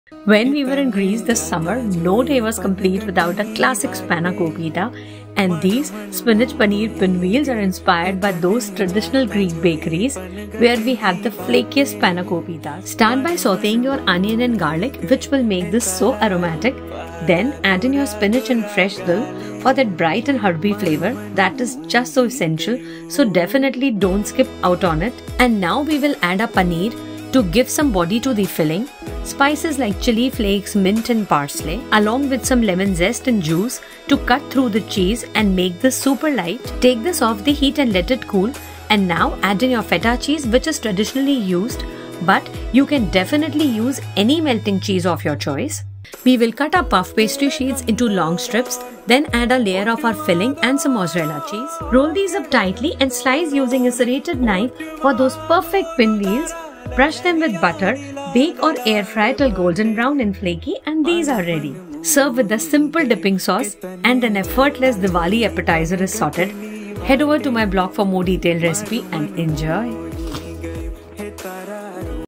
Spinach Paneer Pinwheels
Spinach Paneer Pinwheels — flaky and crisp with a bright, herby filling, these are a perfect Greek-inspired appetizer or snack! 💕

With fresh dill, parsley, mint, and salty feta, enjoy spanakopita in a new form factor with these golden brown pinwheels!✨

NATURALLY NIDHI - vegetarian + food stories + recipes